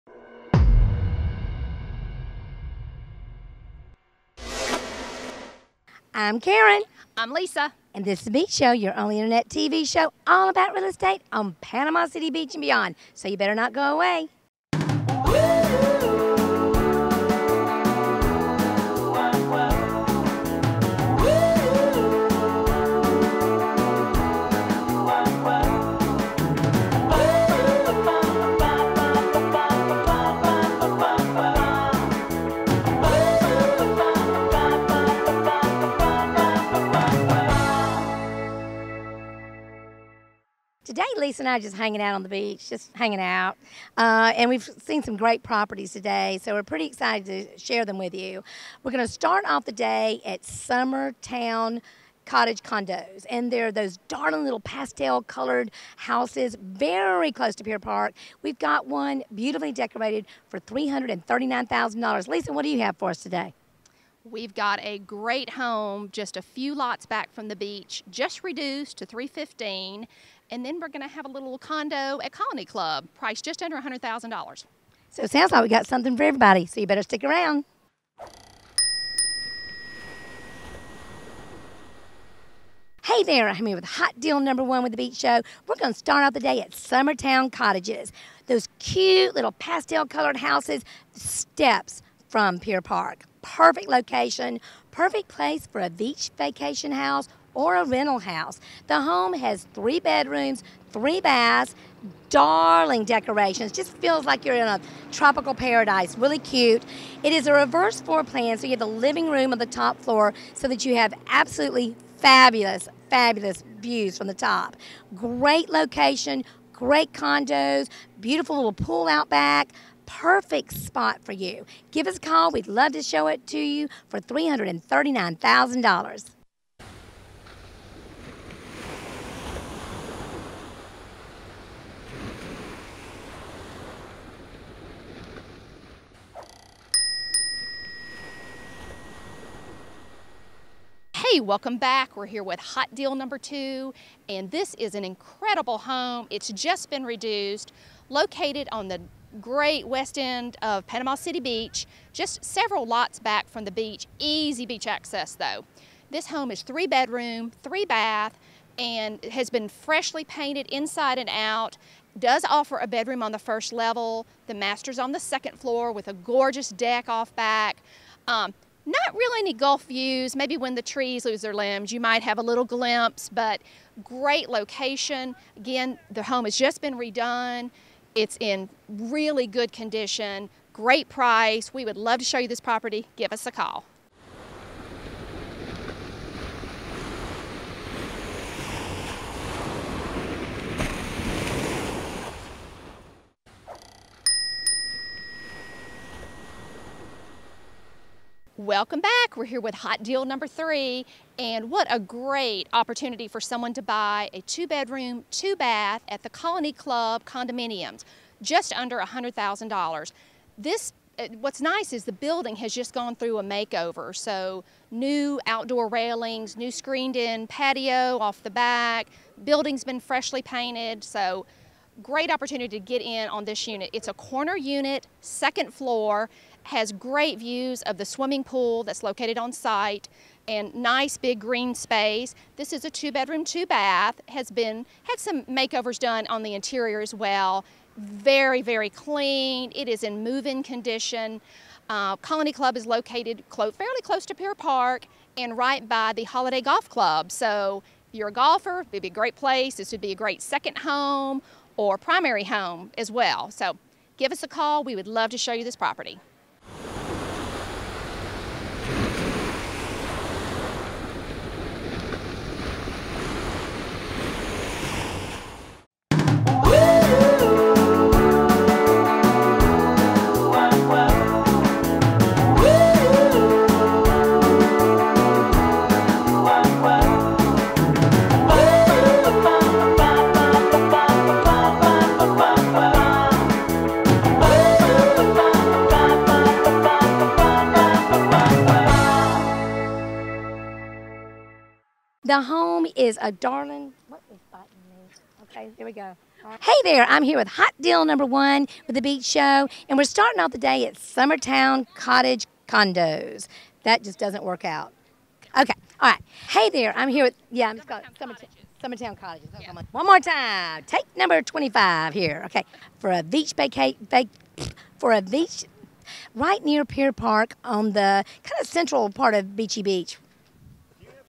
Beach Show #183 - Panama City Beach, Florida Real Estate
Summer Towne Cottage Condo - $339,000

These properties RARELY come on the market, right next to Pier Park
3 Bed/3-1/2 Bath with reverse floor plan taking advantage of great views!
Rental ready (w/few exceptions), freshly painted, community pool.

Great family home on the desirable West End just one block to beach - $315,000

Just reduced - fresh interior paint, large kitchen, tile/wood throughout
Nice fenced corner lot, large 2-car garage, offers 1st floor bedroom
Anderson impact resistant windows, metal roof, newer construction (2003)

Leisure Condo (located at Colony Club) - priced to sell at $99,700

Well maintained & updated 2bed/2bath overlooking golf course/pool
New screened in enclosure on balcony, spacious bedrooms, large closets
Building just underwent a facelift, nicely kept grounds, lots of green space.